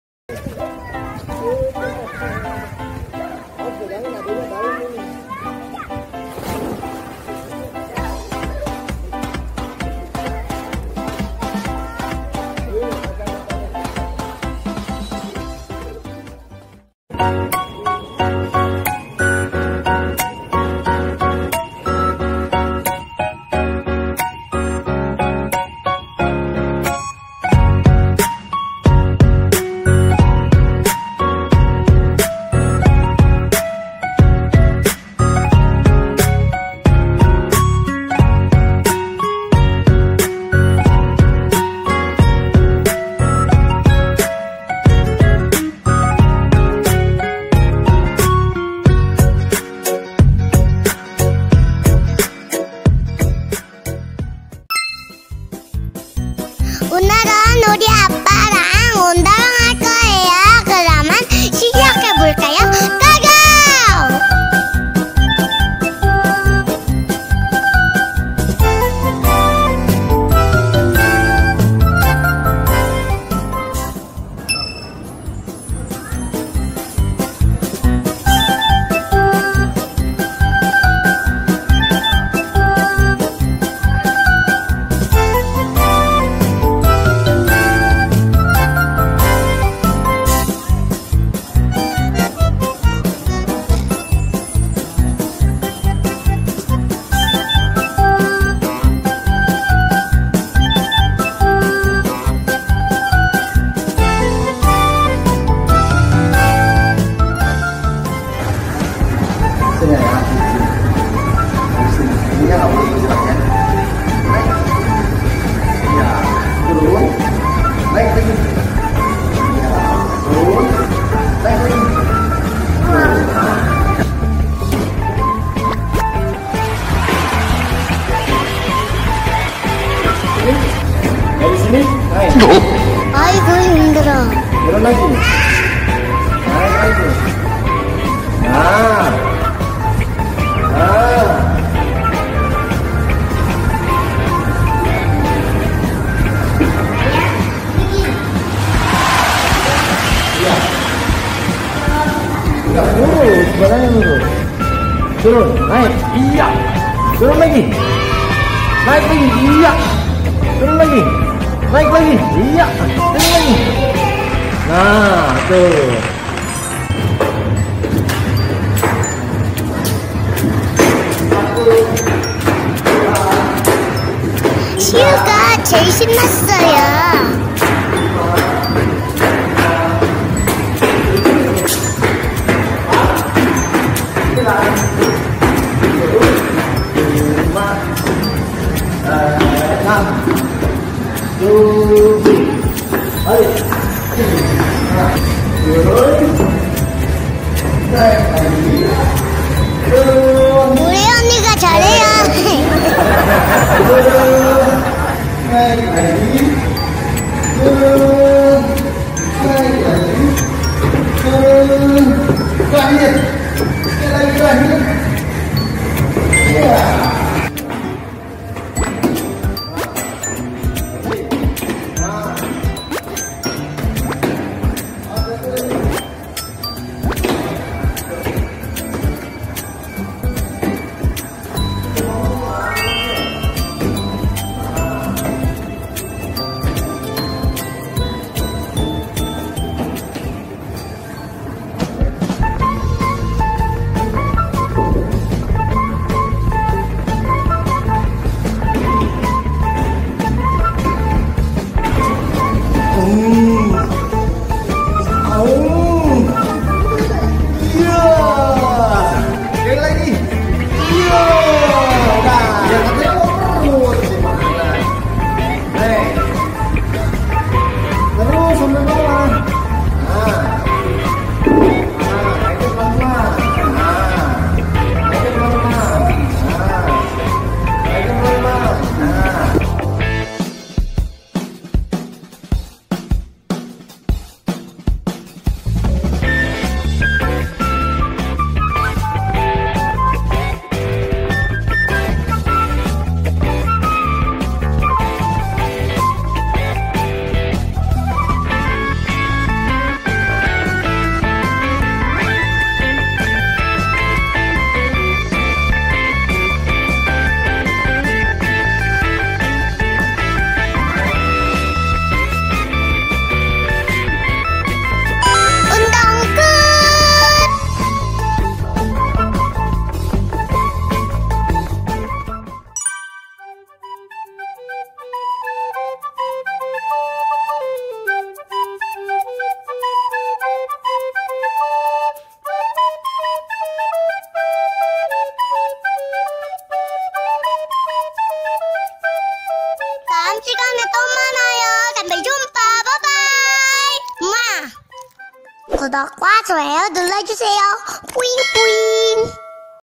Rayu Diary Eps 7 Staycation with family | Part 2 Gym Day
Gym Day while staying in hotel. New experience to do gym with daddy!😄 Jiyu, Rayu and Siyu are able follow instruction of daddy or not? Well... Let's have a look!😊